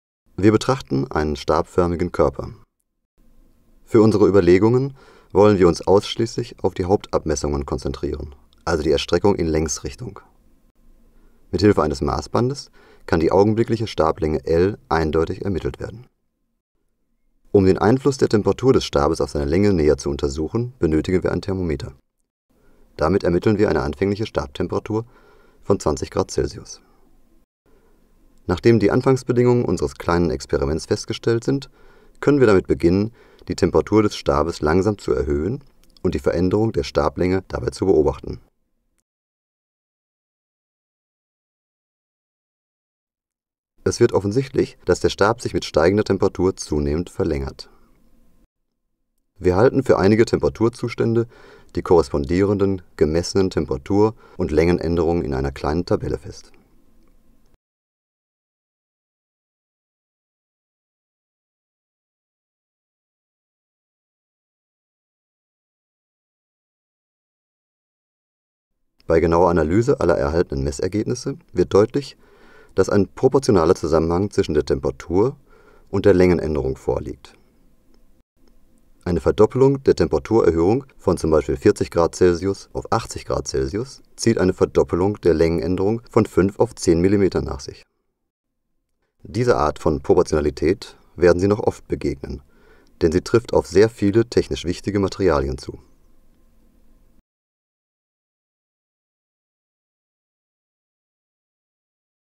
Technische Mechanik - Thermische Dehnung eines Stabes
Diese Animation stammt aus dem Kurs Technische Mechanik im Online Wirtschaftsingenieurwesen Fernstudiengang

Den Effekt der thermischen Dehnung können wir an einem einfachen Stab leicht nachvollziehen. Erwärmt man einen Stab, so beobachtet man eine Ausdehnung in Richtung seiner Längsachse, wie es in der Animation gezeigt ist.